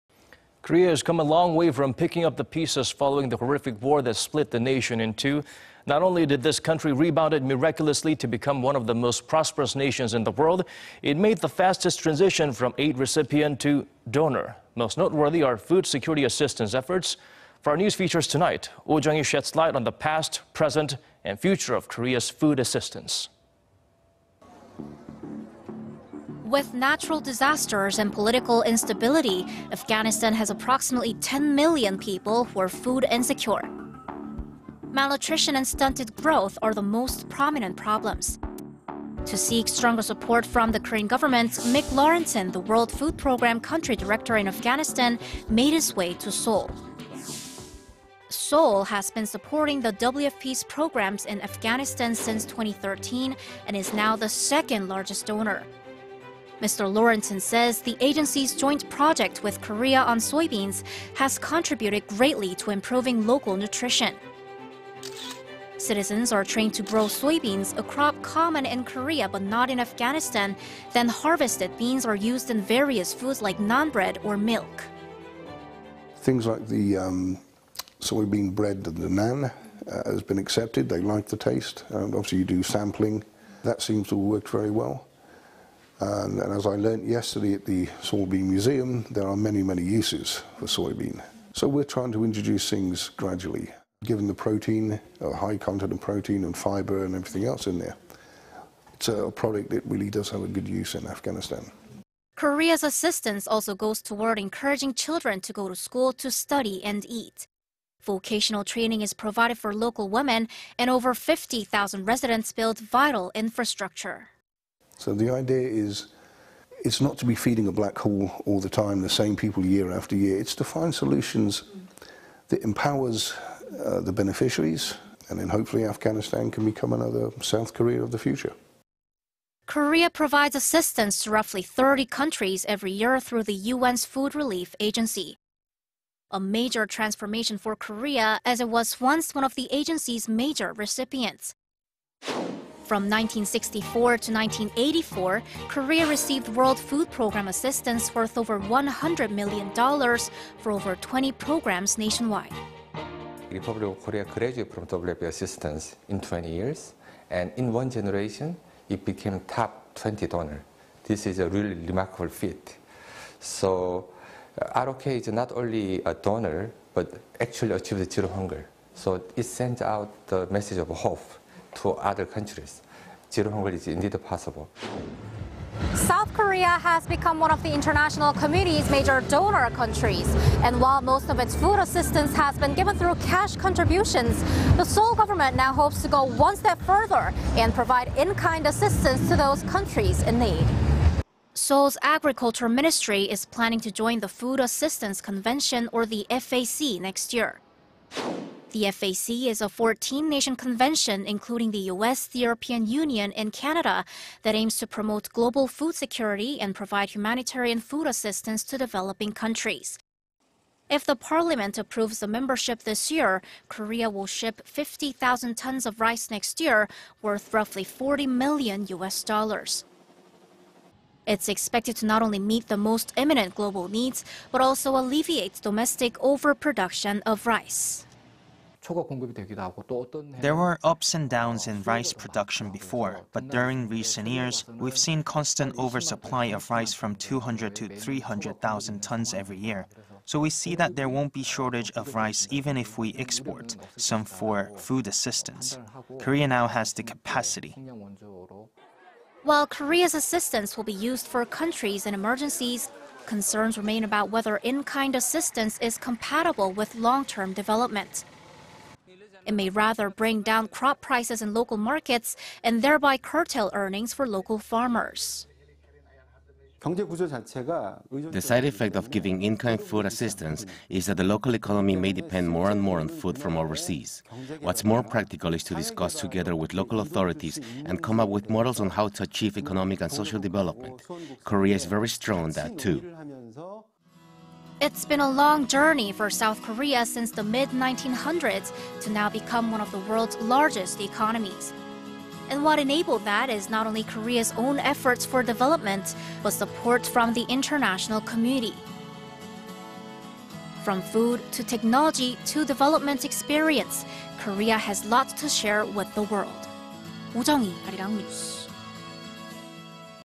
S. Korea becomes one of major donors; hopes to provide rice to developing countries
식량원조국가로서의 대한민국

  Korea has come a long way from picking up the pieces... following the horrific war that split the nation in two.
Not only did this country rebounded miraculouly to become one of the most prosperous nations in the world... it made the fastest transition from aid recipient to donor.
Most noteworthy are food security assistance efforts.
For our news features tonight, Oh Jung-hee sheds light on the past, present, and future of Korea's food assistance.
With natural disasters and political instability,... Afghanistan has approximately 10-million people who are food-insecure.
   Malnutrition and stunted growth are the most prominent problems.
To seek stronger support from the Korean government, Mick Lorentzen, the World Food Programme Country Director in Afghanistan made his way to Seoul.

  Seoul has been supporting the WFP's programs in Afghanistan since 2013... and is now the second largest donor.
Mr. Lorentzen says... the agency's joint project with Korea on soybeans... has contributed greatly to improving local nutrition.
Citizens are trained to grow soybeans, a crop common in Korea but not in Afghanistan,... then harvested beans are used in various foods like naan bread or milk.

(English)
    "(   )Things like the soybean bread, the naan, has been accepted, they like the taste. (   ) After you do sampling, that seems to work very well. And as I learned yesterday at the soybean museum, there are many many uses of soybean. So we're trying to introduce it gradually. Given the protein and high fiber and everything else in there, it's a probably.. really does have a big use in Afghanistan."

Korea's assistance also goes toward encouraging children to go to school... to study and eat.
Vocational training is provided for local women... and over 50-thousand residents build vital infrastructure.

(English)
    "So the idea is... (     )it's not to be feeding a black hole all the time, the same people year after a  year, it's to find solutions that empowers the beneficiaries...(   ) and hopefully Afghanistan can become another South Korea in the future."

  Korea provides assistance to roughly 30 countries every year through the UN's food relief agency.
A major transformation for Korea... as it was once one of the agency's major recipients.
 From 1964 to 1984, Korea received World Food Programme assistance worth over 100-million dollars for over 20 programs nationwide.

(English)
    "ROK graduated from WFP's assistance in 20 years and in one generation,  it became top 20 donor. This is a remarkable feat. So ROK's not only a donor,  but actually achieved Zero Hunger. So it sends out the message of hope to other countries, Zero Hunger is indeed possible."

(Stand-up)
  “South Korea has become one of the international community’s major donor countries. And while most of its food assistance has been given through cash contributions,... the Seoul government now hopes to go one step further,... and provide in-kind assistance to those countries in need."

Seoul's agriculture ministry is planning to join the Food Assistance Convention, or the FAC, next year.
 The FAC is a 14-nation convention including the U.S., the European Union, and Canada... that aims to promote global food security and provide humanitarian food assistance to developing countries.
  If the parliament approves the membership this year,... Korea will ship 50-thousand tons of rice next year... worth roughly 40-million U.S. dollars.
It's expected to not only meet the most imminent global needs... but also alleviate domestic overproduction of rice.